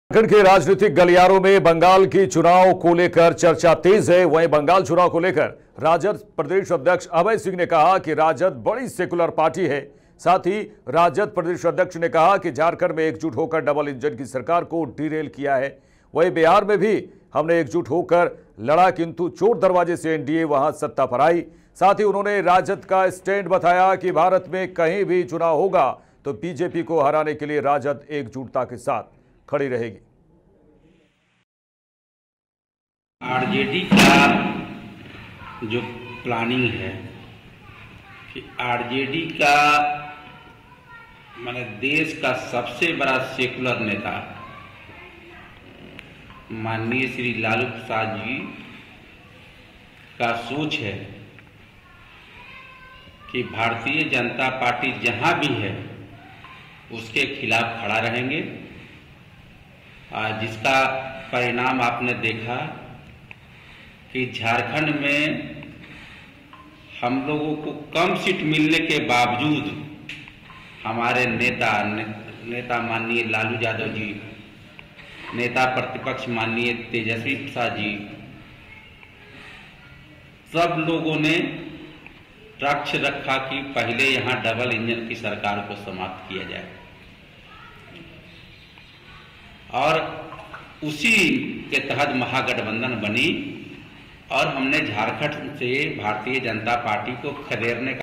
Ranchi : बंगाल चुनाव से पहले राजनीति तेज, बीजेपी को हराने के लिए राजद का ये है प्लान, देखें वीडियो
बंगाल चुनाव से पहले राजनीति तेज, बीजेपी को हराने के लिए राजद का ये है प्लान, देखें वीडियो